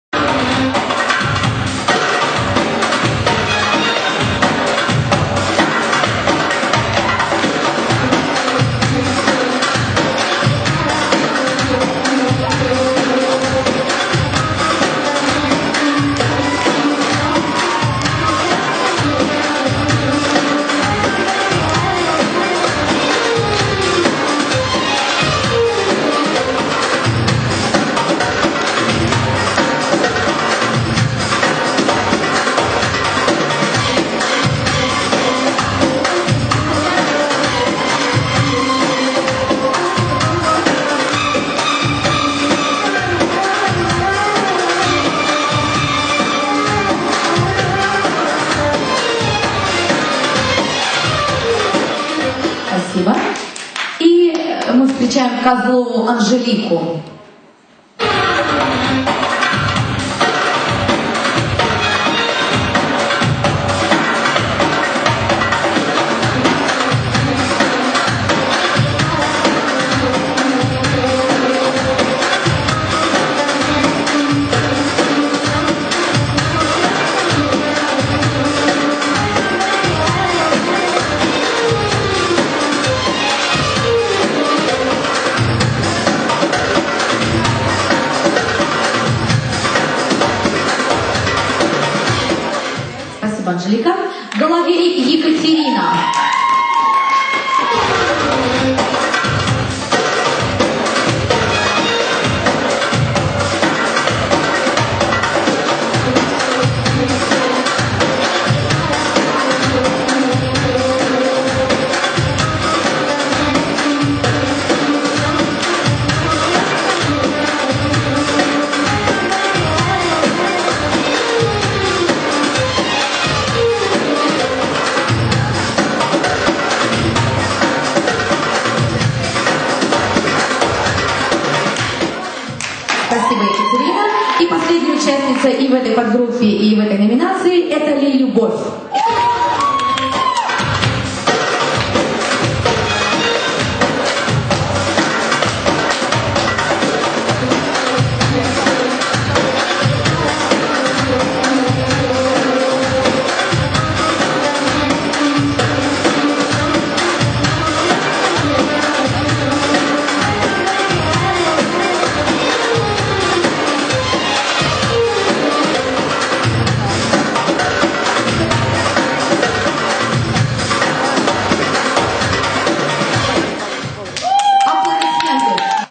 Отборочный (Козлова А., Головей Е., Ли Л.), ОТБОРОЧНЫЙ ТУР
4 МЕЖДУНАРОДНЫЙ КУБОК ВОСТОЧНОГО ТАНЦА. МОСКВА 11-12 ОКТЯБРЯ 2008